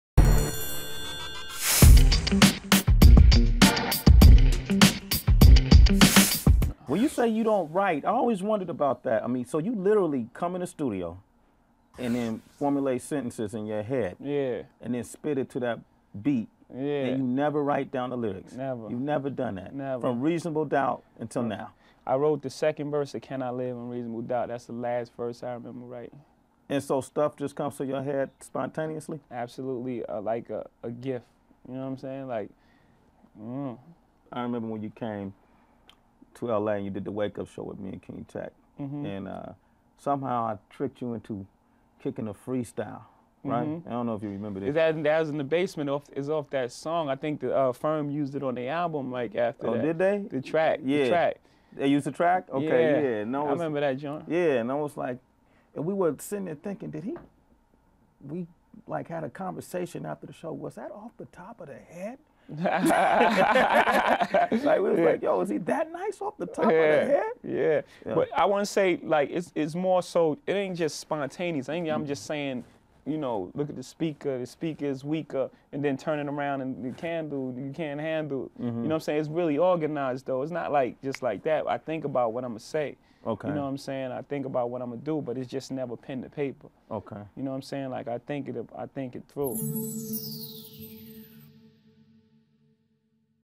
Jay-Z and Sway - Explains Not Writing Anything Down, Recalls Freestyling for Sway - 2003
sway asks about hovs writing process

interview